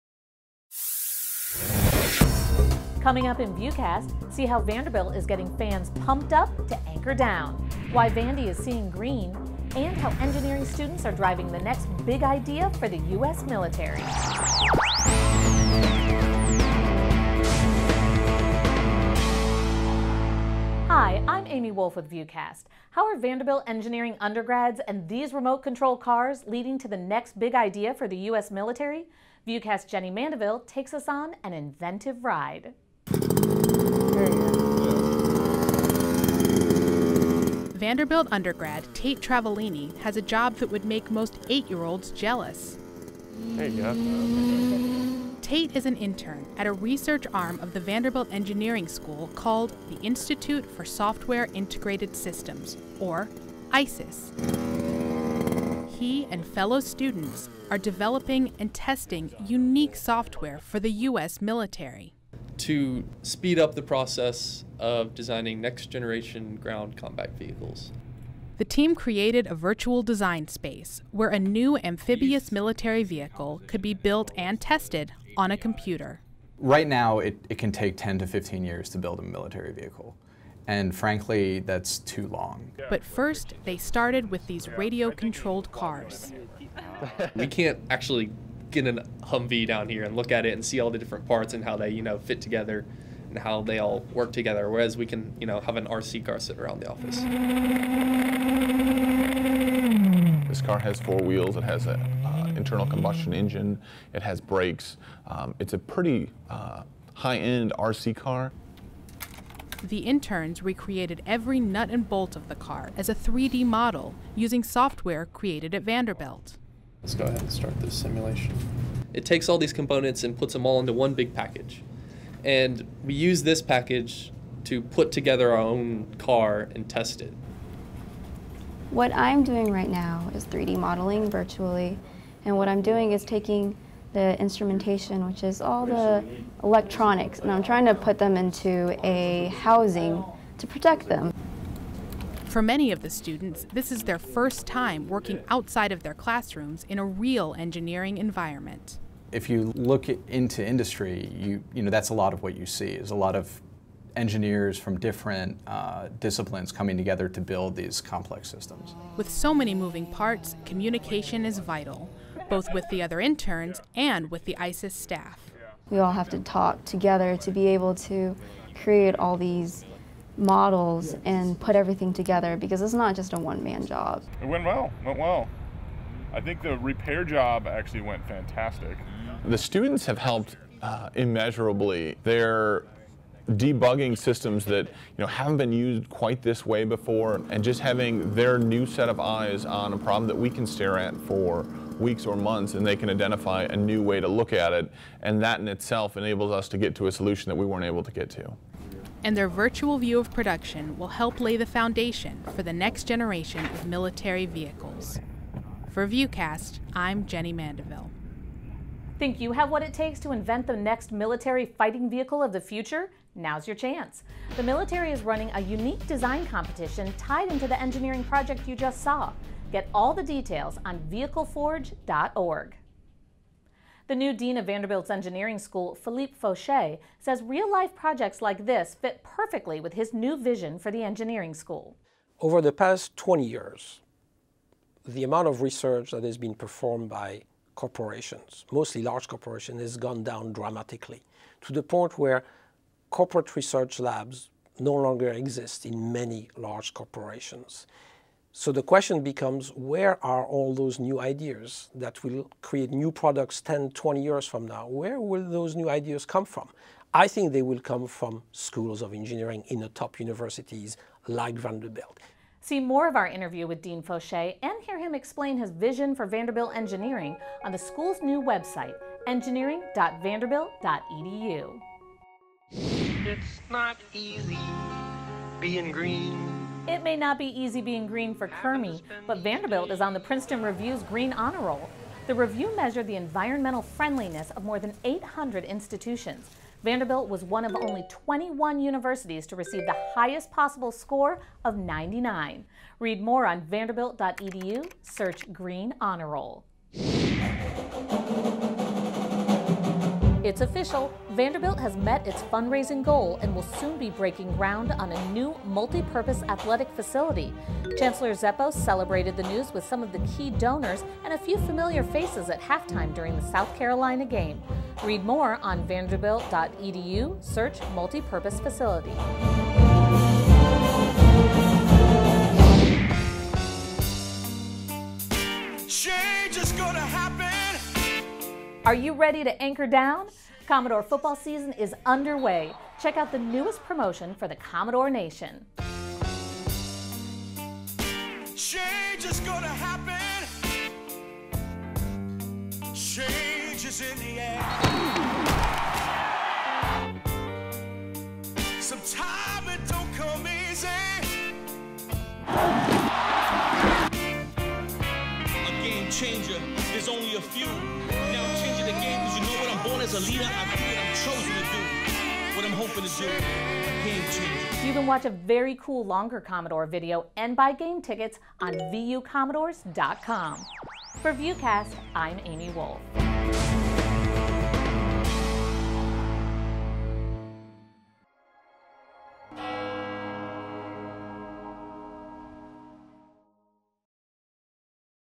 VUCast Newscast: Driving Design
This Week on VUCast, Vanderbilt's online newscast:
·         How engineering students are driving the next big idea for the U.S. Military.
·         Why is Vanderbilt seeing green?
·         See how Vanderbilt is getting fans pumped up to Anchor Down!